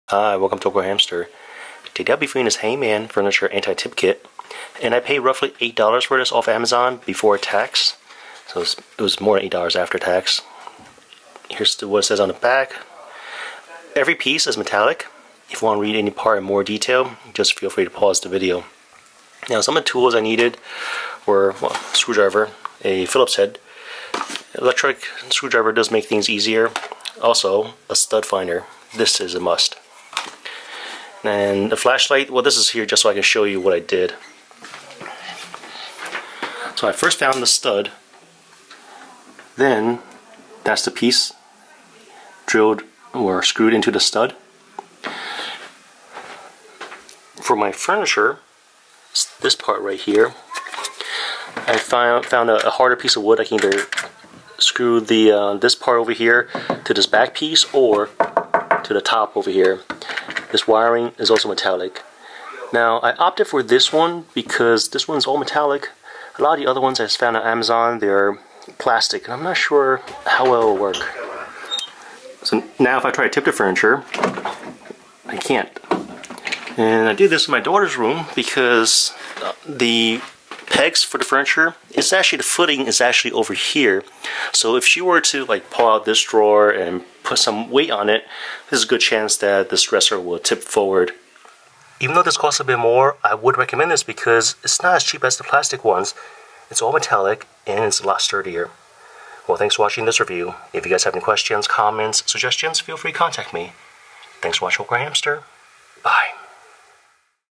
Hangman Furniture Anti Tip Prevention Steel Cable Kit TK-400 Review
Holds up to 400lbs.
Helps prevent injury for children and adults
Helps prevent furniture, filing cabinets, and full length mirrors from falling over.
Tamper resistant
Made of high grade galvanized aircraft steel cable and steel components